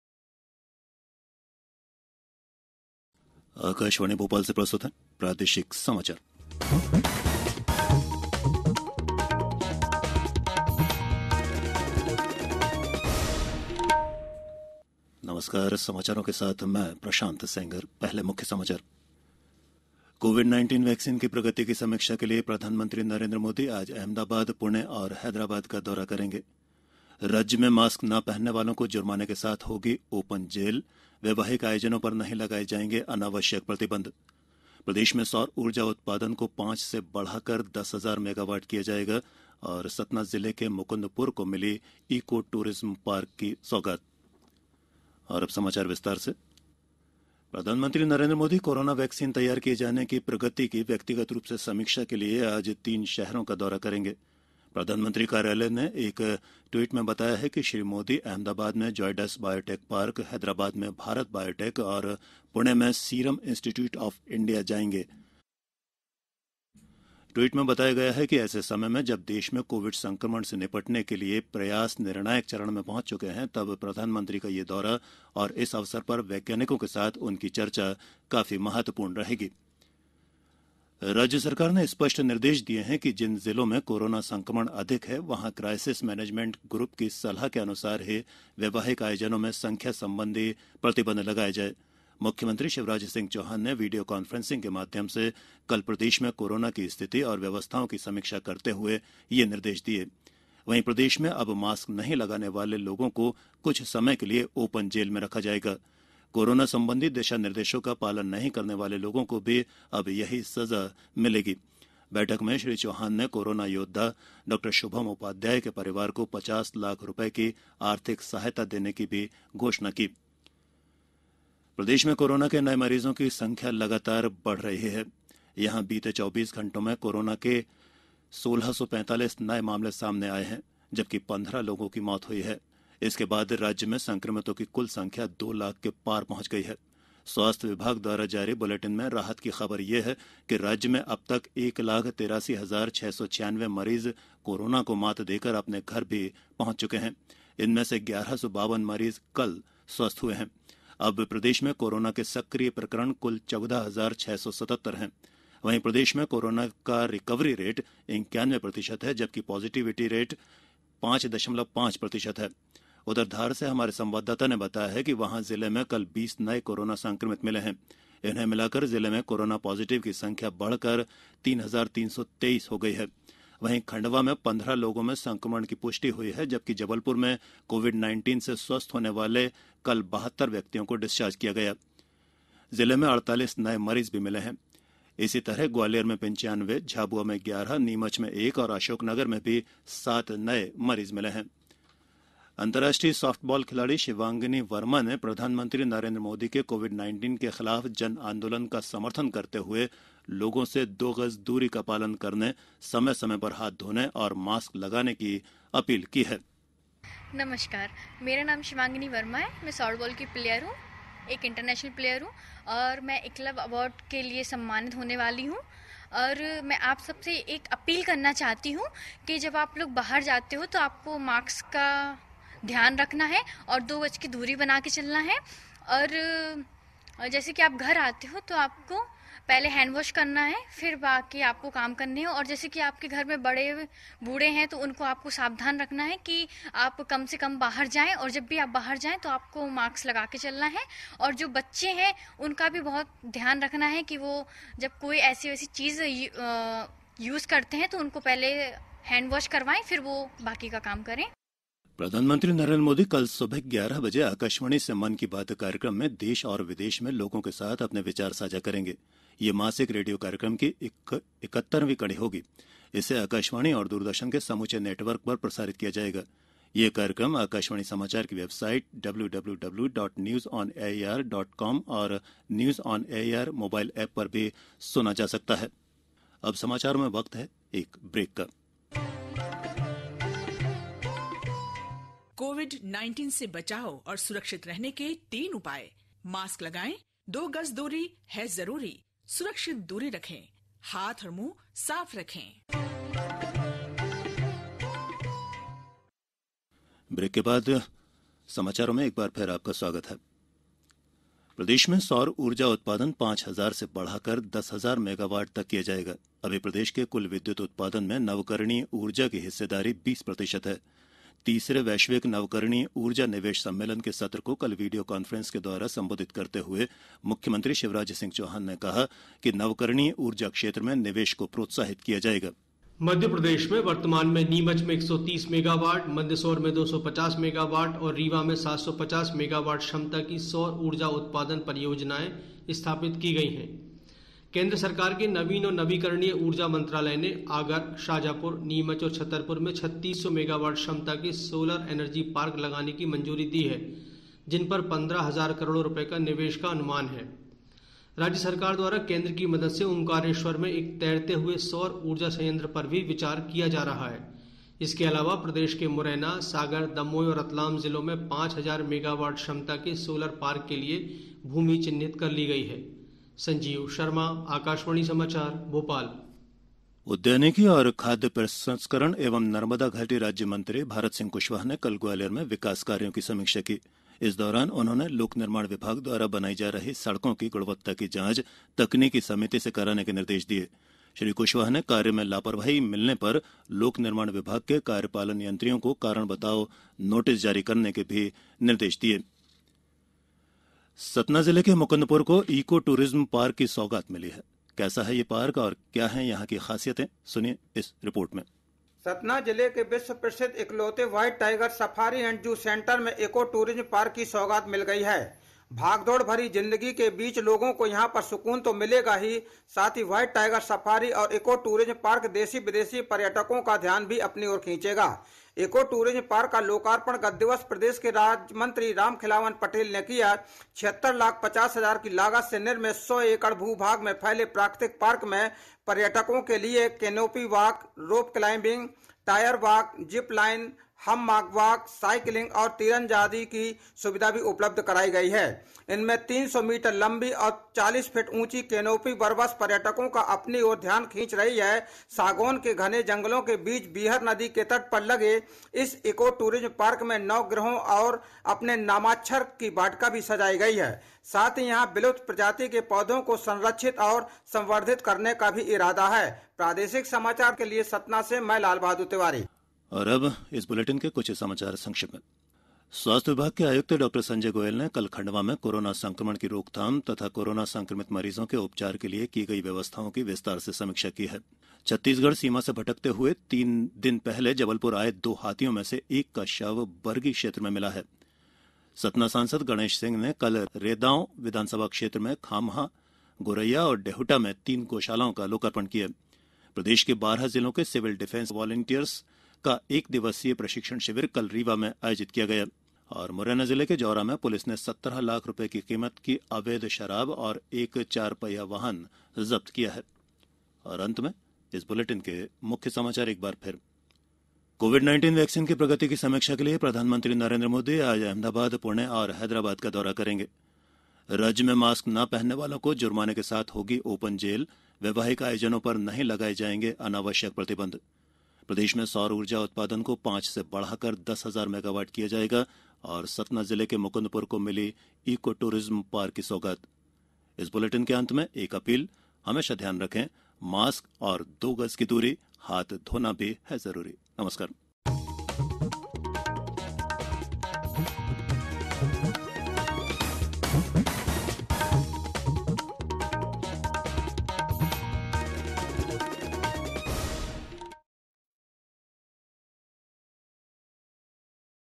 news 28 11 20, 7 10 am
कोविड-19 वैक्सीन की प्रगति की समीक्षा के लिए प्रधानमंत्री नरेन्‍द्र मोदी आज अहमदाबाद, पुणे और हैदराबाद का दौरा करेंगे।

राज्य में मास्क न पहनने वालों को जुर्माने के साथ होगी ओपन जेल, वैवाहिक आयोजनों पर नहीं लगाए जाएंगे अनावश्यक प्रतिबंध।
प्रदेश में सौर ऊर्जा उत्पादन को 5 से बढ़ाकर 10 हजार मेगावाट किया जाएगा।